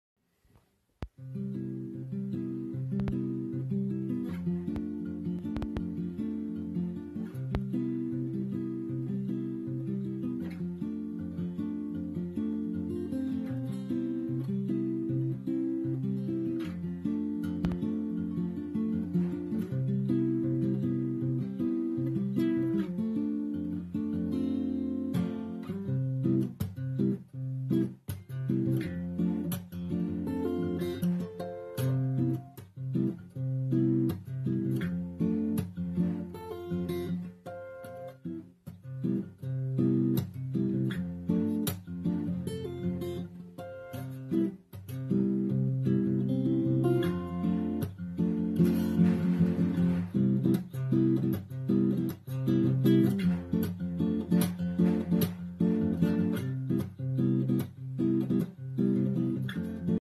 Frigid Friday Fingerstyle Fun
Fun with Fingerstyle on a Frigid Friday. Feeling the inspiration from two guitar greats, Mark Knopfler and Steven Wilson. Guild acoustic, open Eb tuning, capo 2nd fret.